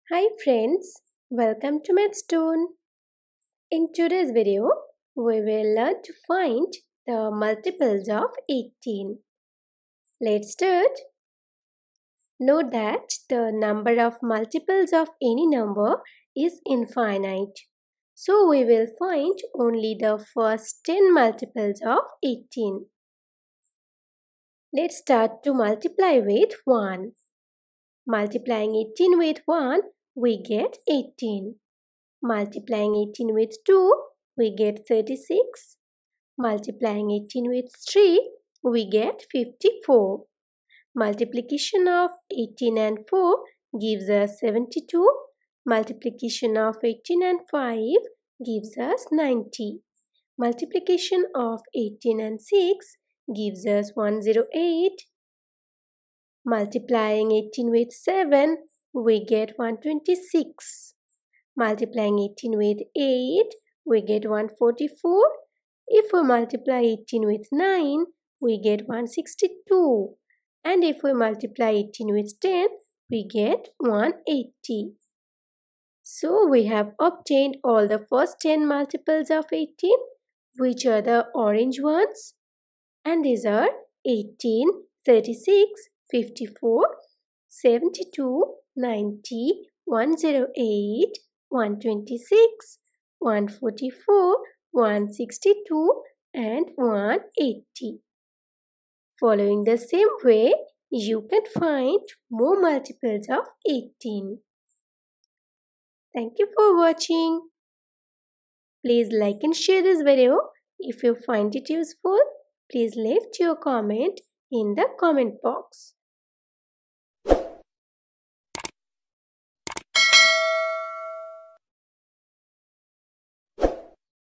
Multiples of 18 | Multiplication table of 18
In this video, we will find the multiples of 18 in a very easy way.
Topic: Multiple of 18 | Multiply by 18 | 18 times table | Multiplication table of 18

Question: What are the first ten multiples of 18?
Answer: The first ten multiples of 18 are 18, 36, 54, 72, 90, 108, 126, 144, 162, and 180.

We post videos daily on youtube